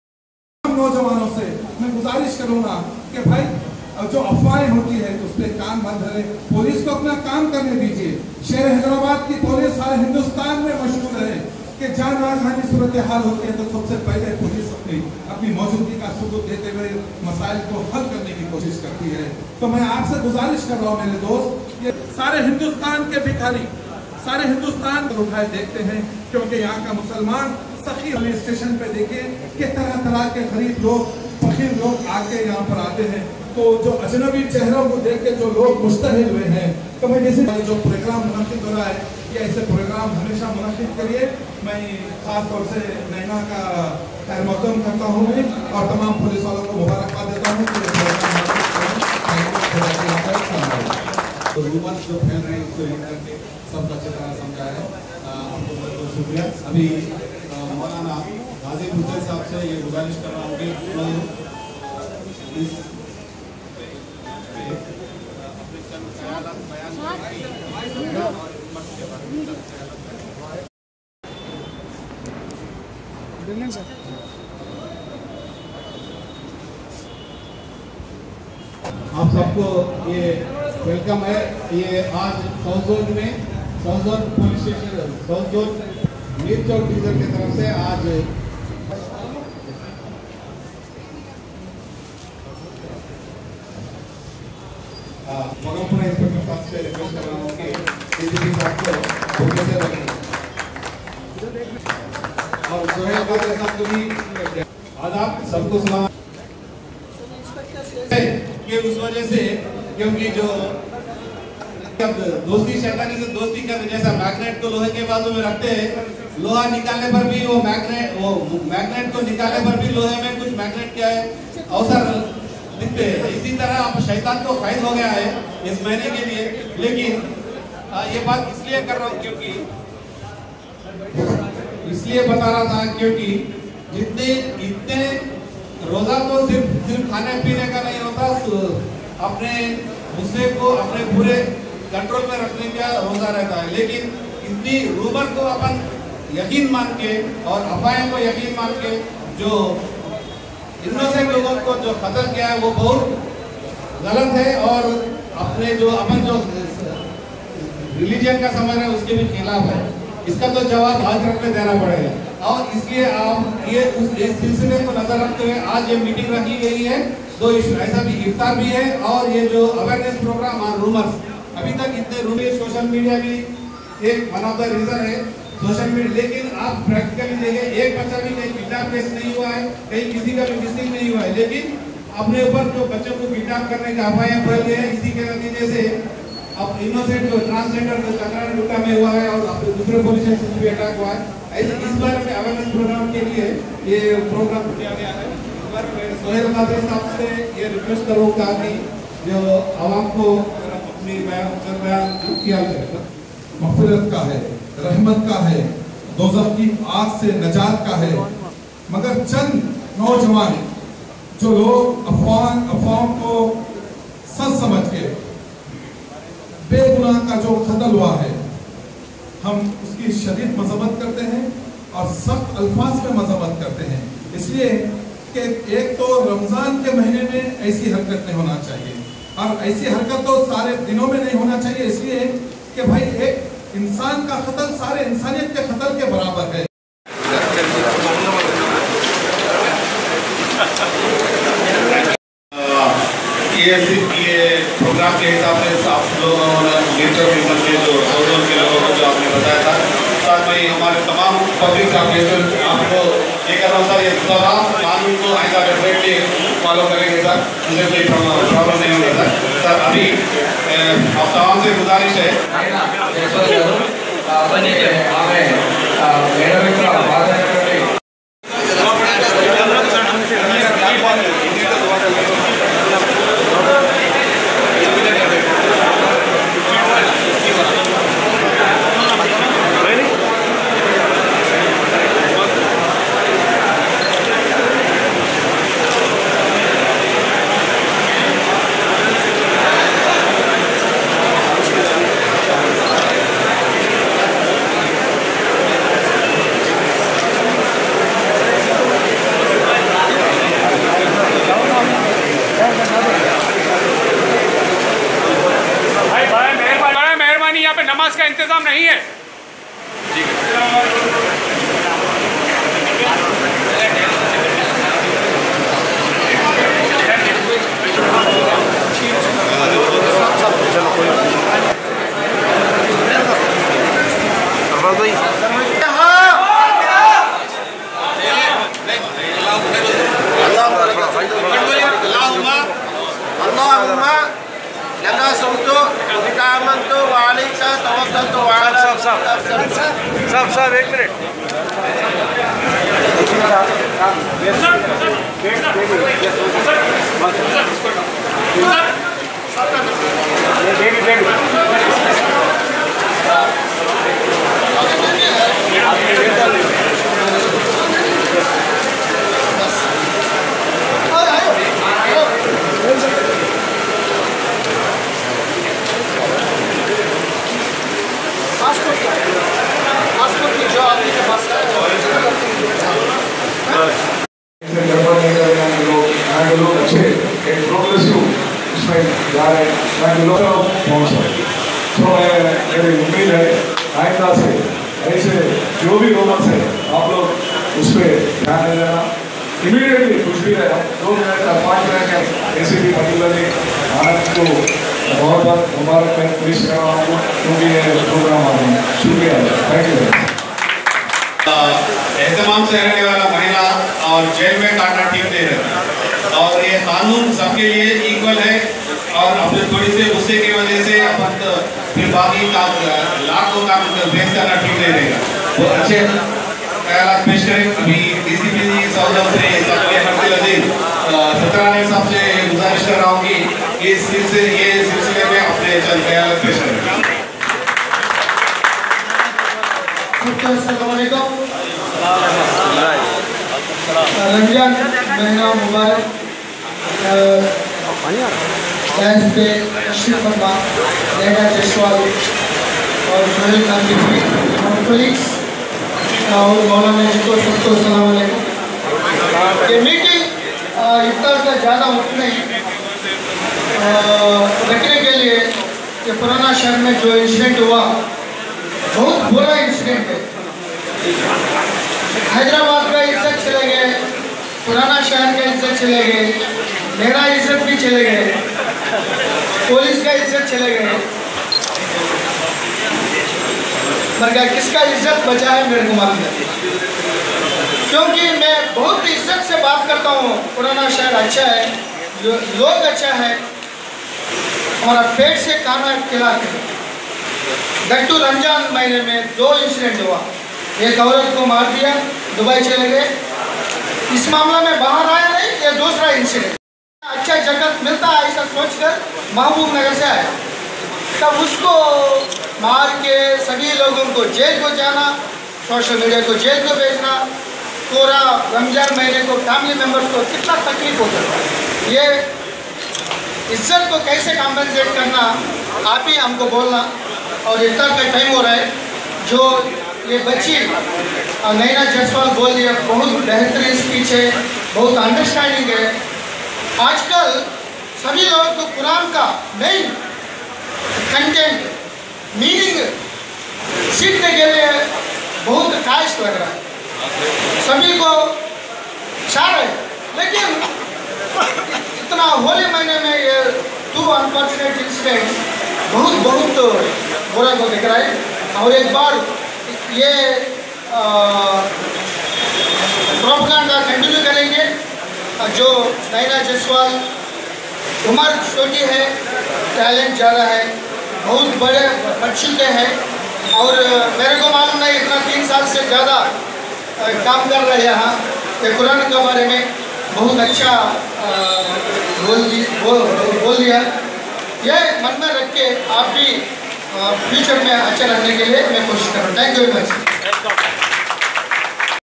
Mirchowk Division hosted Dawat E Iftaar at Gulzar Function hall. Dcp South Zone V Satnarayana...
Mirchowk Division hosted Dawat E Iftaar at Gulzar Function hall